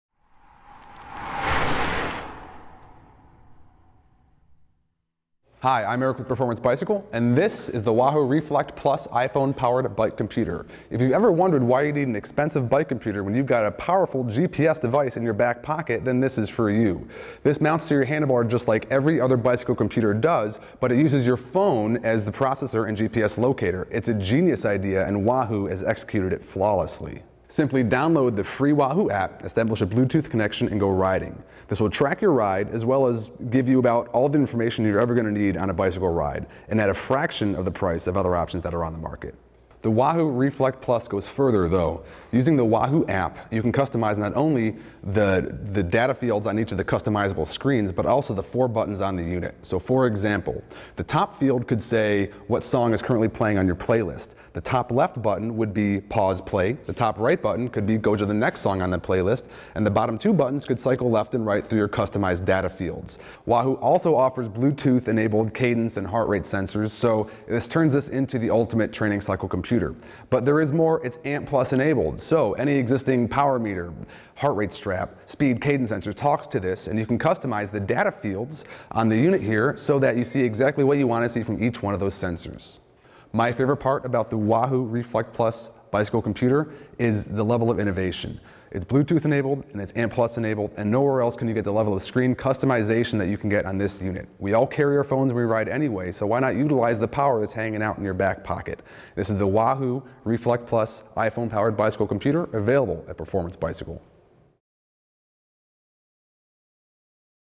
Wahoo Fitness RFLKT iPhone Powered Bike Computer Review by Performance Bicycle
The RFLKT shows GPS, heart rate, cadence and other sensor data, plus accesses all the other features of your iPhone, such as music, internet, social media and more. Customize the way data displays on the screen, build pages to view while training, listen to your music library, send commands to your cycling App -- all powered by your iPhone. The RFLKT manages all your App data from the handlebars, so you can safely stow your iPhone in jersey pocket or backpack. Welcome to iPhone powered cycling.